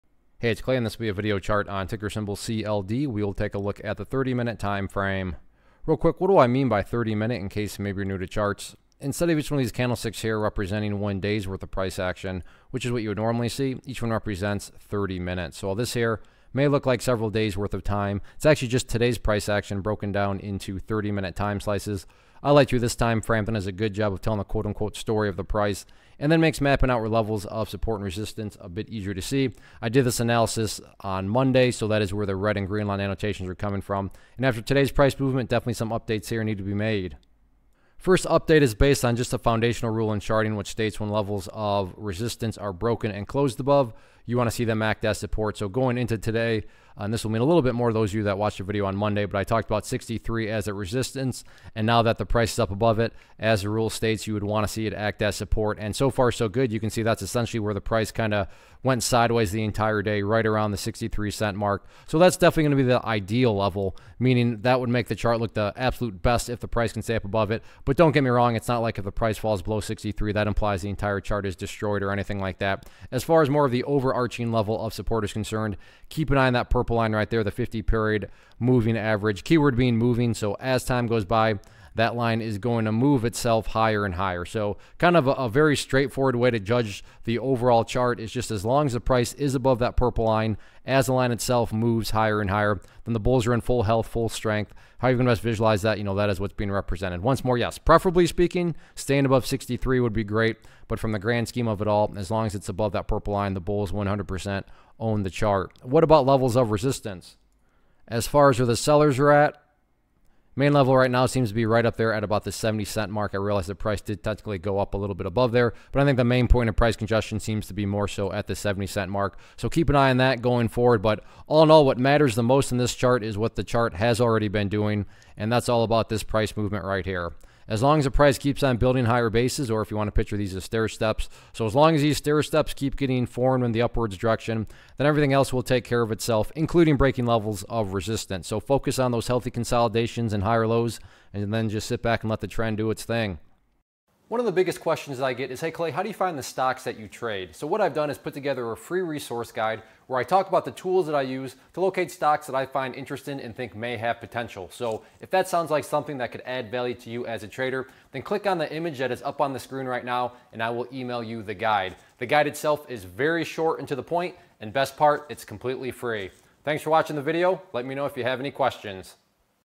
Cloud Peak Energy Inc. - CLD Stock Chart Technical Analysis for 02-12-2019
Learn how to read stock charts and identify technical patterns as ClayTrader does a quick stock chart review on Cloud Peak Energy Inc. (CLD).